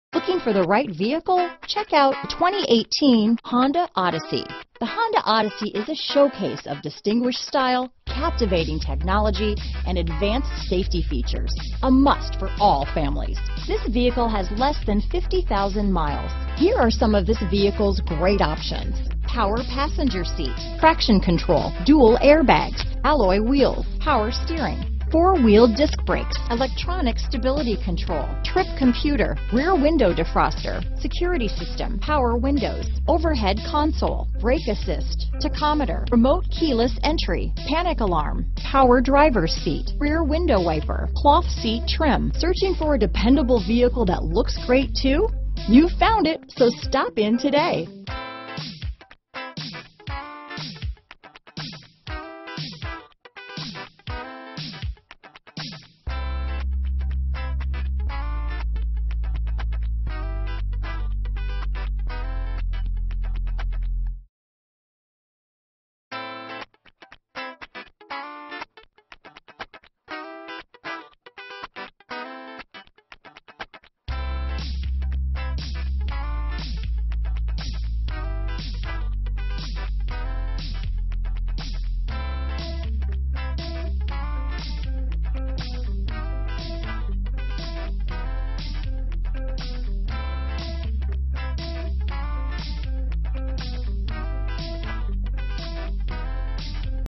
2018 Honda Odyssey Columbia, Lexington, Irmo, West Columbia, Aiken, SC UC2818
Modern Steel Metallic Used 2018 Honda Odyssey available in Columbia, South Carolina at Honda of Columbia. Servicing the Lexington, Irmo, West Columbia, Aiken, SC area.

2018 Honda Odyssey LX 3.5L V6 SOHC i-VTEC 24V 19/28 City/Highway MPG Certified. Clean CARFAX. CARFAX One-Owner.